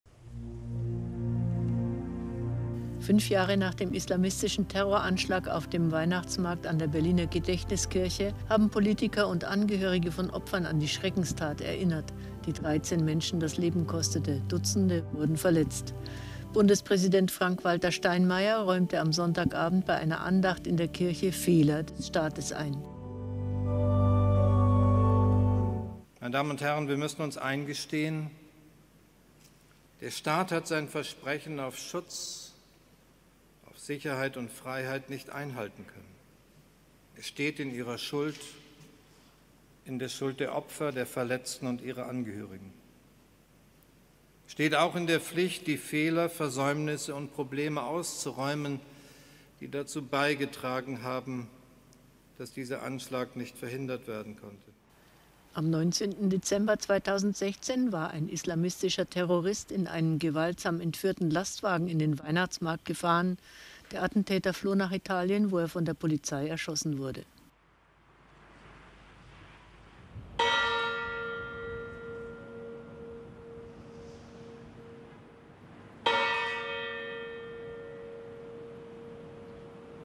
Gedenken: 5 Jahre Anschlag am Breitscheid-Platz - "Staat steht in Ihrer Schuld"
"Der Staat hat sein Versprechen auf Schutz, auf Sicherheit und Freiheit nicht einhalten können," so Bundespräsident  Frank-Walter Steinmeier bei der Gedenkfeier für die 13 Opfer in der Berliner Gedächtniskirche.